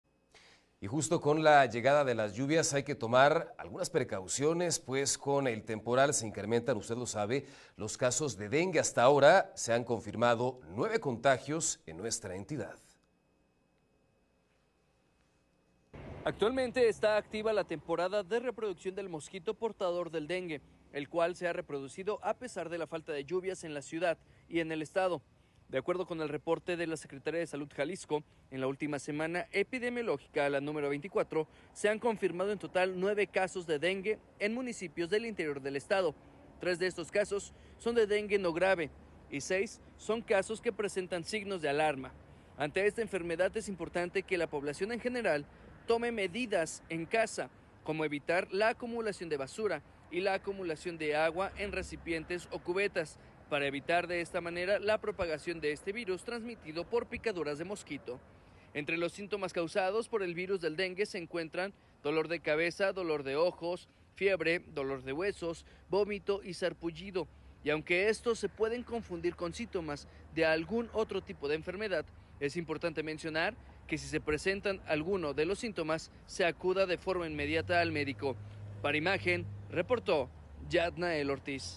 9 casos de dengue confirmados en Jalisco | Imagen GDL con Ricardo Camarena
Con la llegada del temporal de lluvias se activa la temporada del mosquito portador del Dengue. La Secretaría de Salud Jalisco confirmó nueve casos en municipios al interior del estado.

ImagenNoticiasGDL con Ricardo Camarena